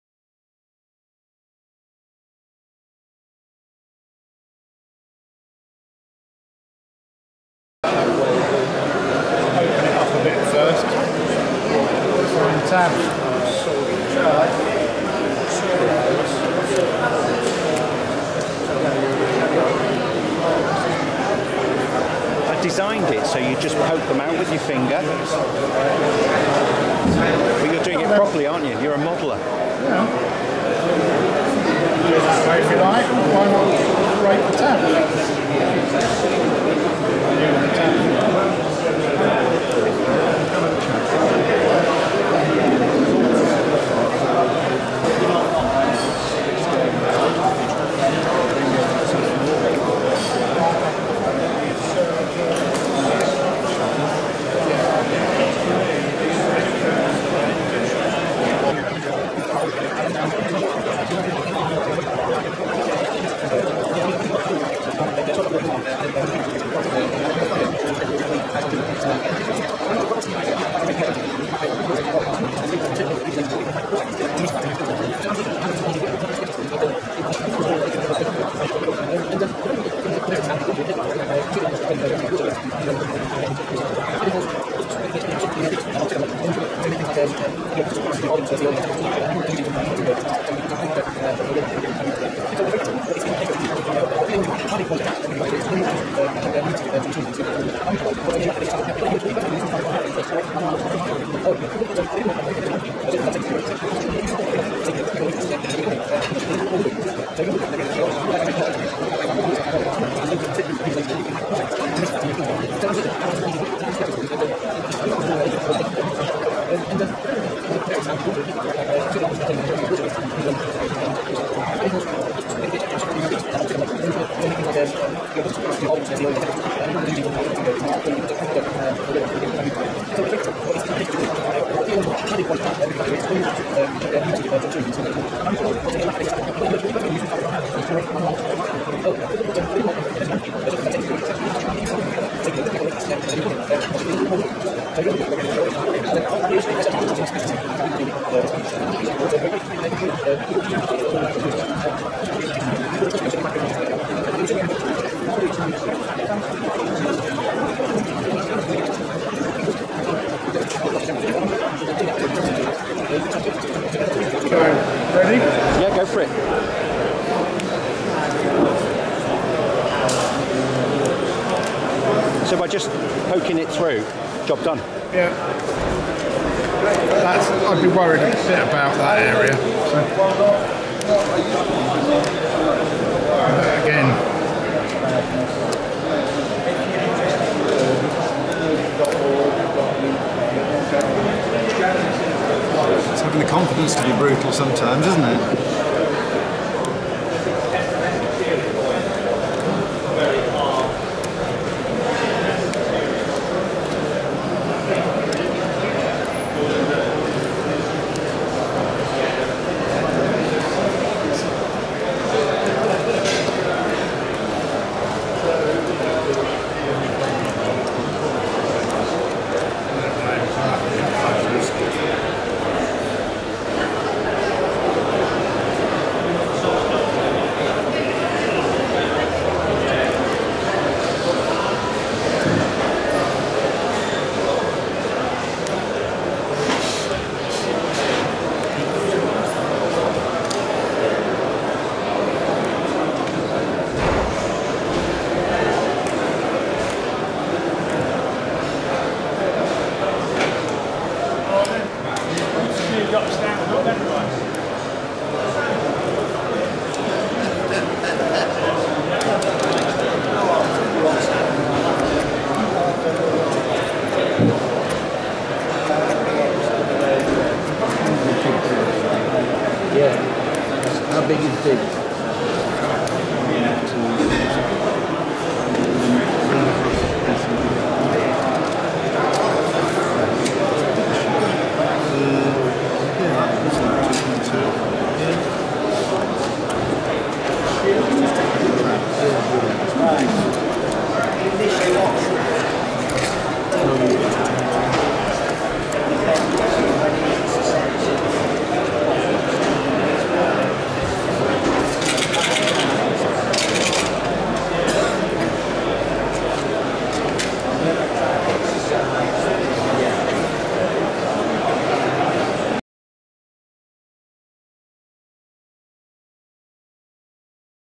Servo Mount Assembly
We spotted Mike Collins from the Leamington and Warwick club assembling our servo mounts at the International N Gauge exhibition in 2019.

Seems Mike is happy with the mounts as they are destined for the clubs new layout – all controlled using MegaPoints Controllers servo drivers and MultiPanel mimic panel controllers.